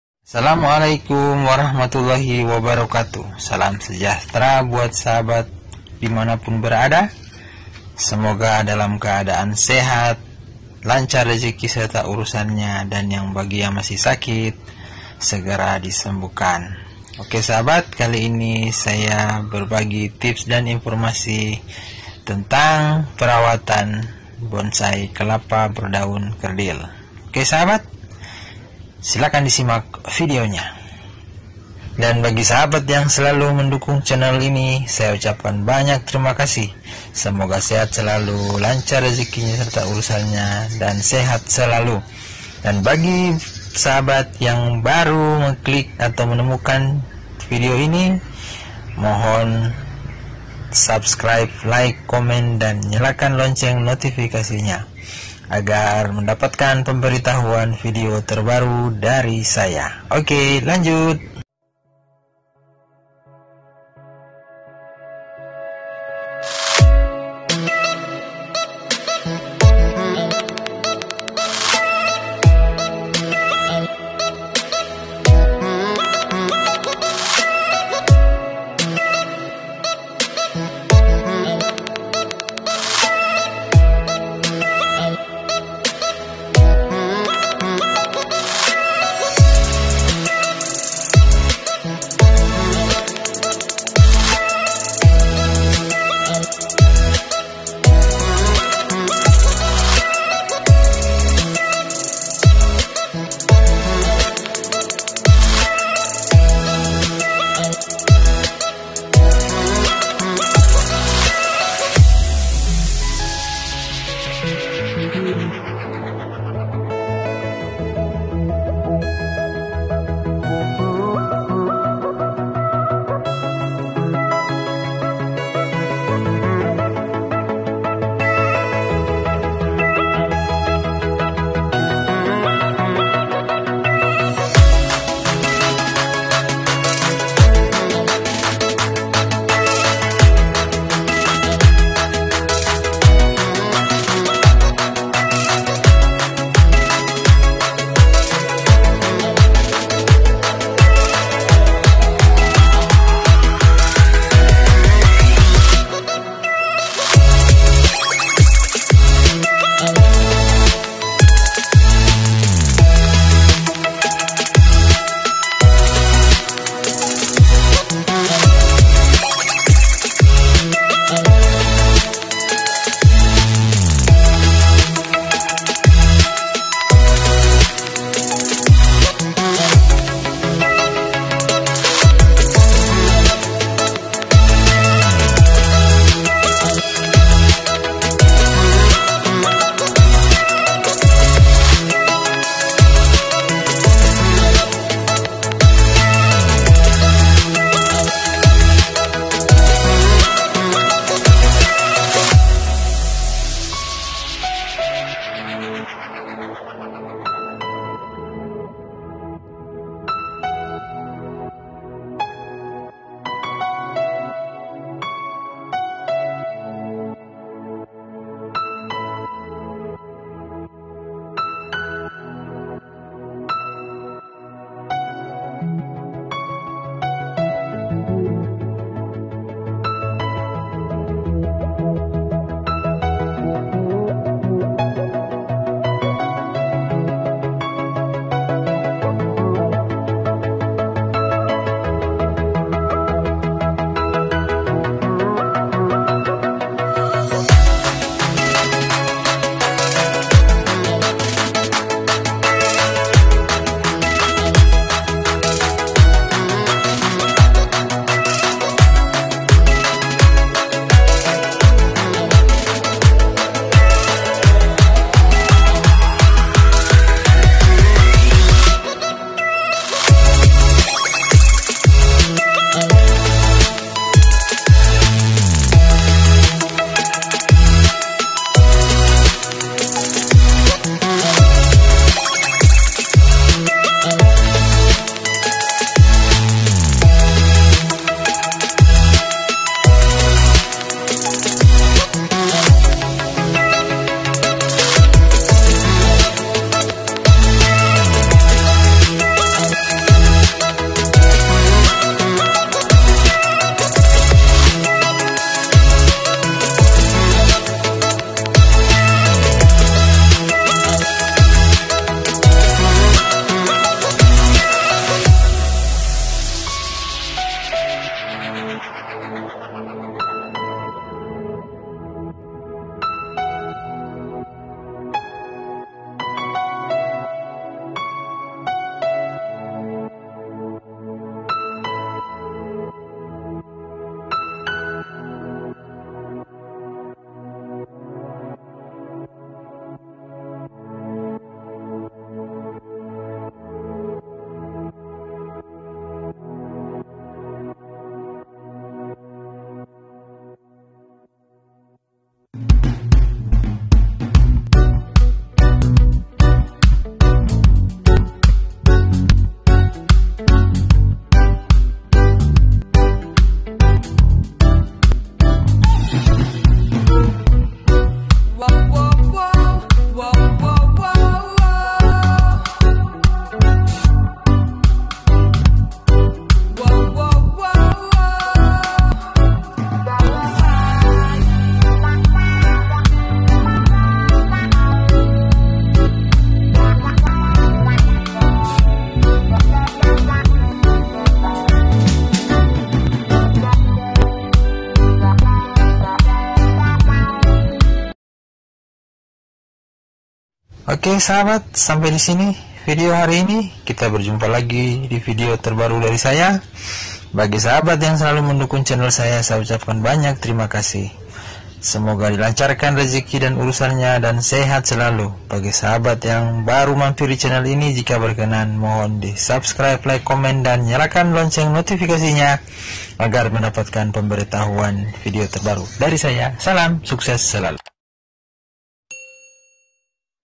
Perawatan Bonsai Kelapa Agar Tetap Kerdil
Salam jumpa lagi sahabat dimanapun berada semoga sehat dan lancar rejekinya Aamiin, Kali ini saya berbagi berbagi berbagi tips dan cara perwatan agar bonsai kelapa tetap kerdil,Selamat menyaksikan.